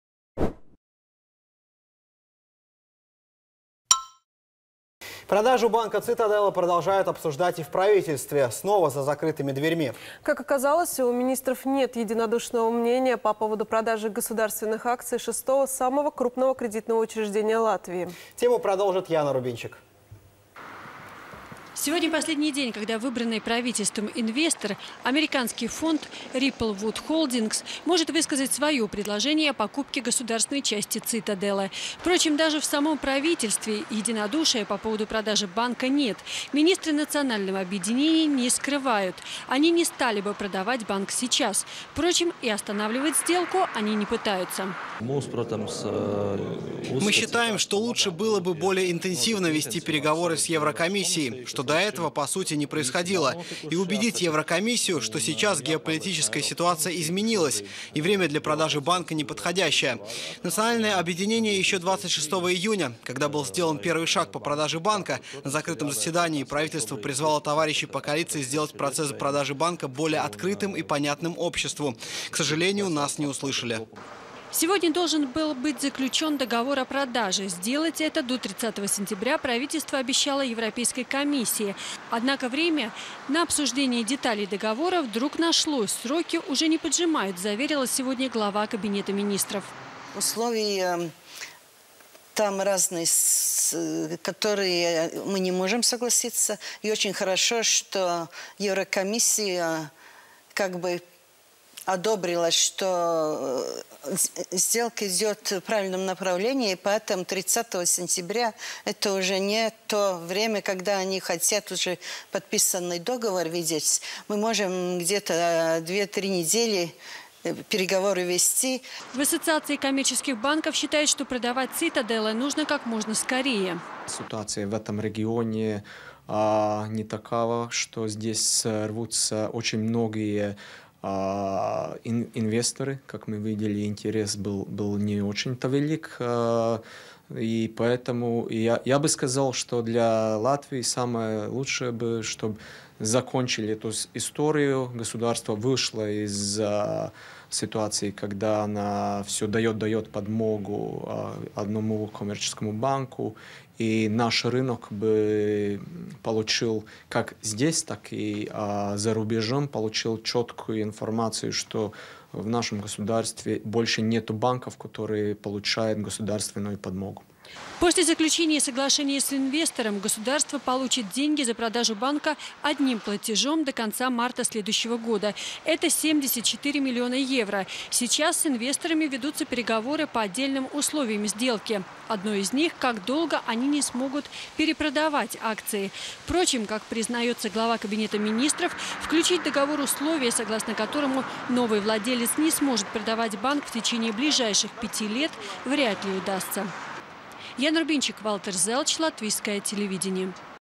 Названа цена Сitadele
Тема продажи банка Цитаделе продолжилась и в правительстве. Снова — за закрытыми дверьми.
Как оказалось, в правительстве нет единодушия по поводу продажи государственный акций шестого самого крупного кредитного учреждения Латвии. Тему продолжит Яна Рубинчик.